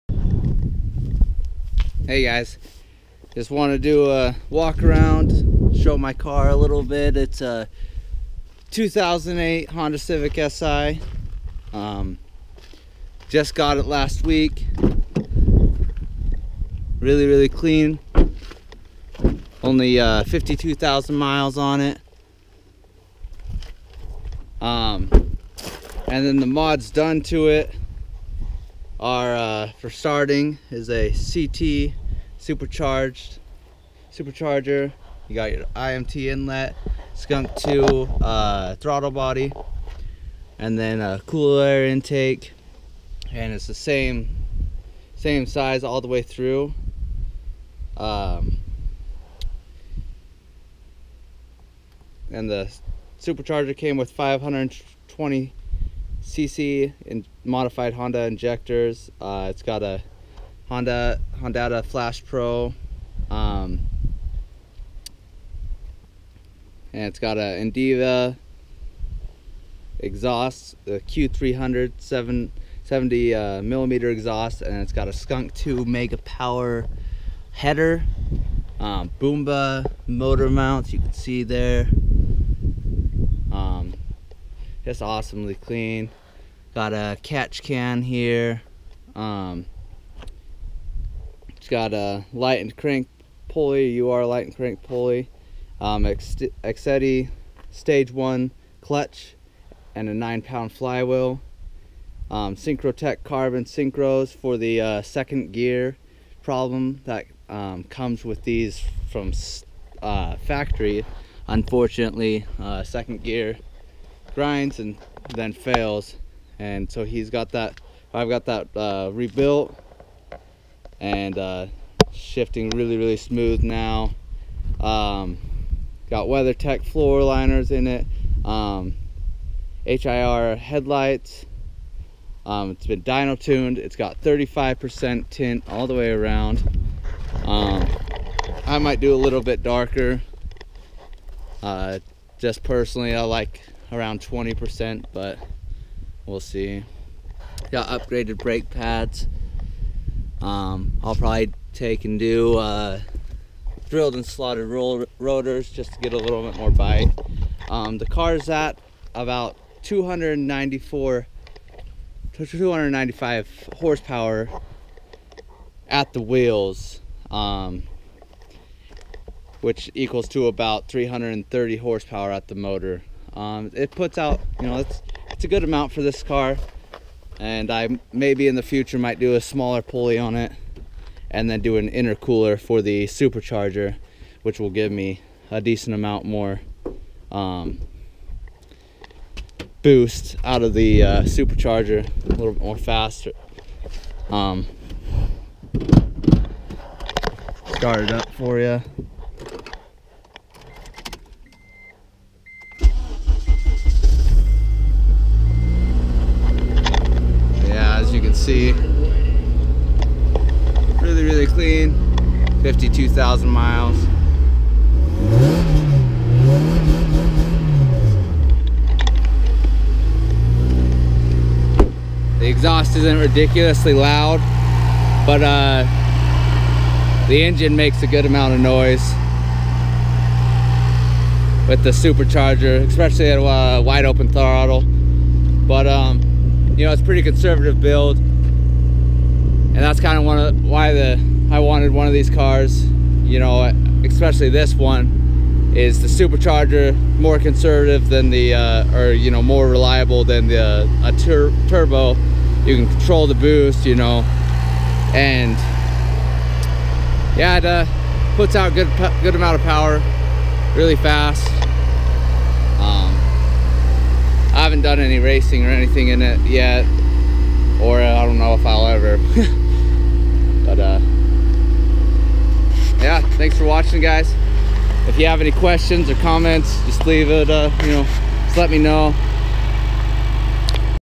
Supercharged Honda Civic
First look at my Si, just got it about a week ago.